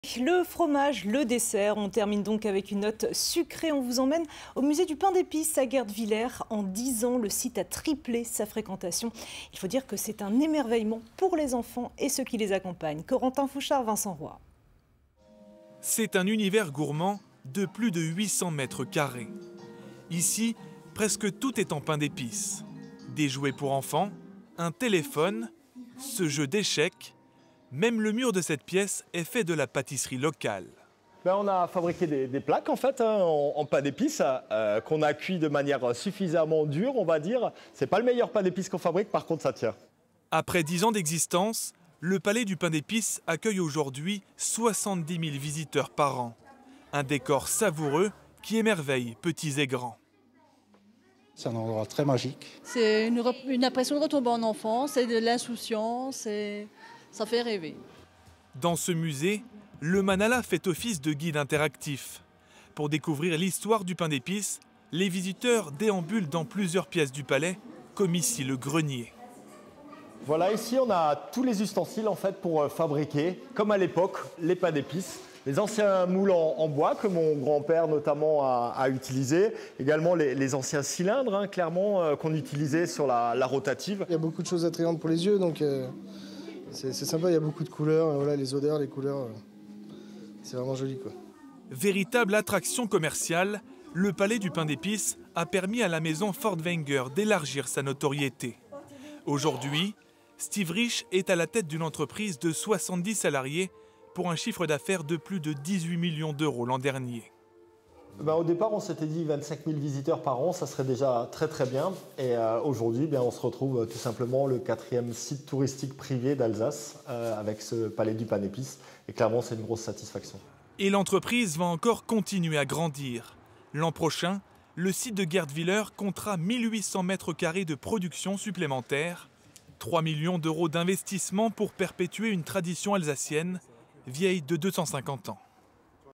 le Palais du pain d'épices de Gertwiller fête ses 10 ans.
Le Palais du pain d'épices fête son 10 eme anniversaire. Véritable musée consacré à ces gâteaux dont l'origine remonte à l'Antiquité, c'est aussi une excellente vitrine pour l'entreprise Fortwenger le numéro 1 français du pain d'épices.